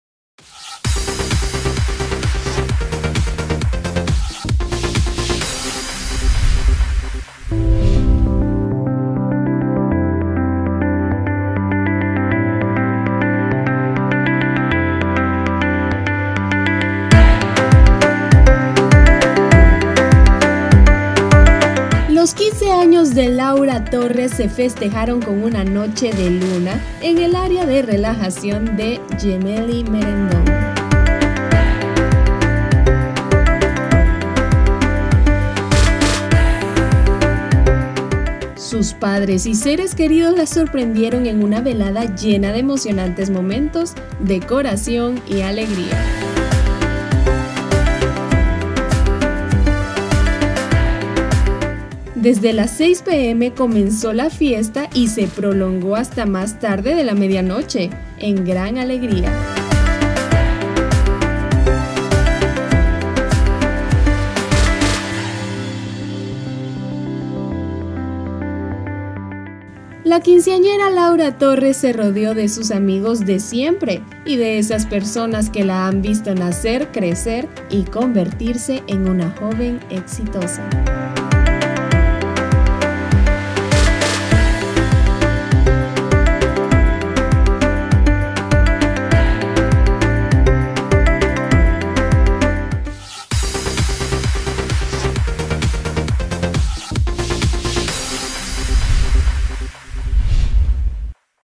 15 años Laura Torres - Iconos IM
Los 15 años de Laura Torres se festejaron con una noche de luna en el área de relajación de Gemelli Merendón!